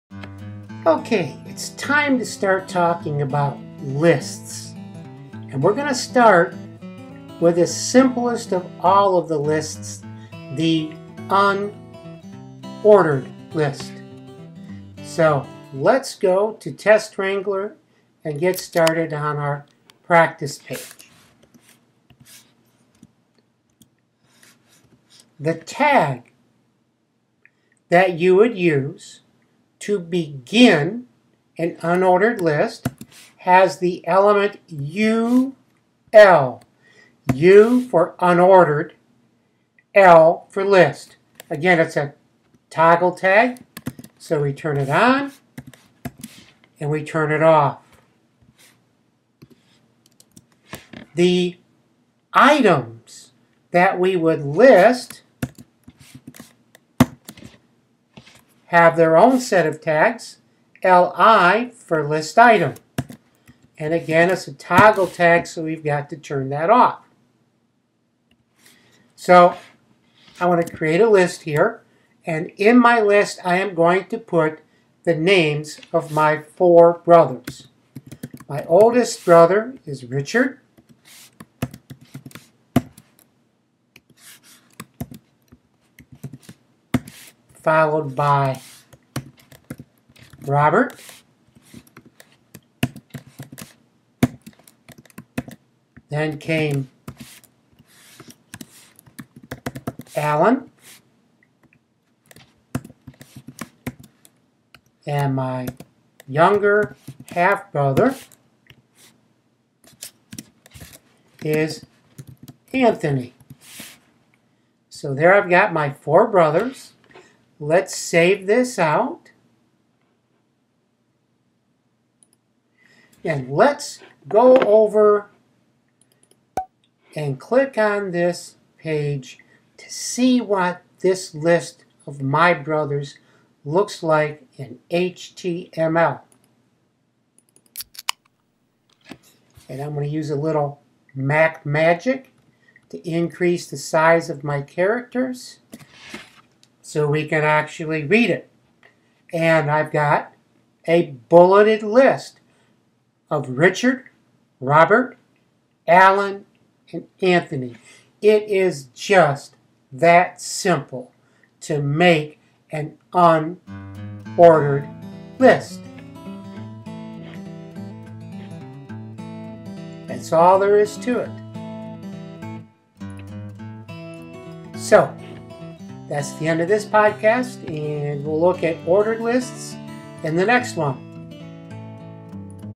HTML: Unordered lists
Dr. Mark shows how to construct an unordered list.

This podcast is a companion resource to w3schools' HTML tutorial Lists

Study when and where you wish by reading the w3schools online tutorials and watching my podcasts. When you finish you will be ready to take the professional certification exam.